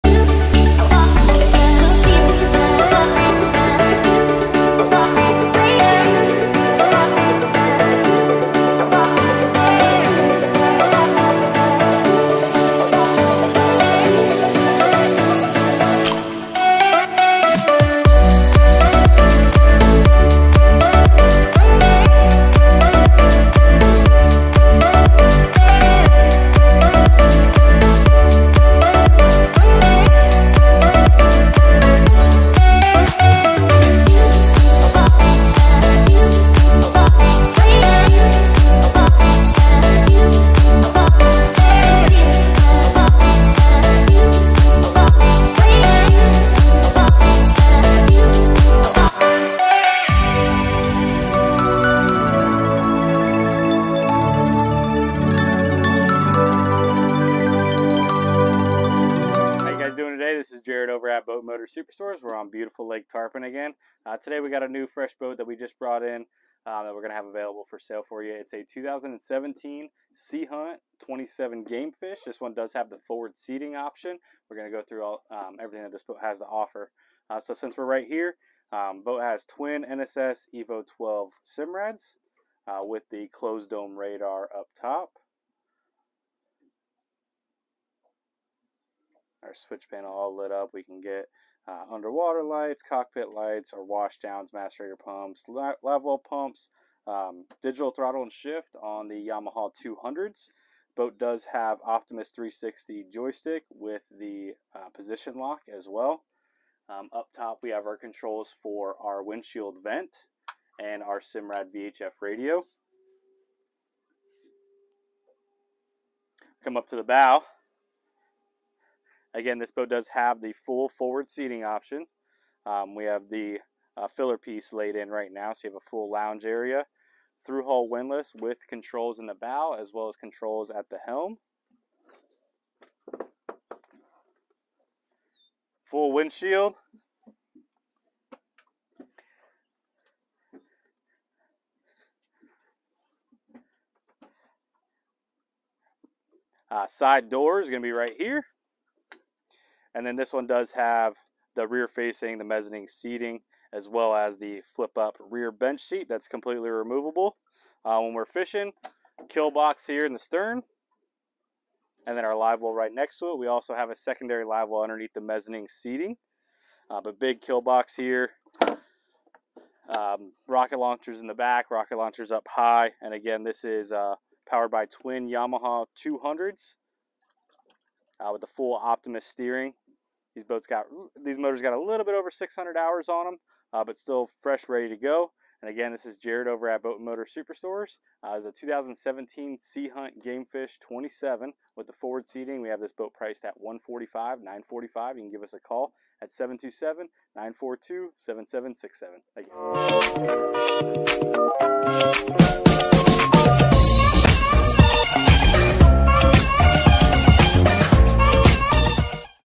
2017 Sea Hunt 27 Gamefish
2017 Sea Hunt 27 Gamefish • $145,945

Hard top with enclosure
Marine deck at helm
Baitwell s
Livewells
Fresh water Washdown
Raw water Washdown
Underwater lights
Windlass
Macerator
Twin Simrad
Windshield vent
Fwd & aft spreader lights
Infinity stereo
Removable rear seating
Leaning post Tackle storage
Porto Potti
Simrad radar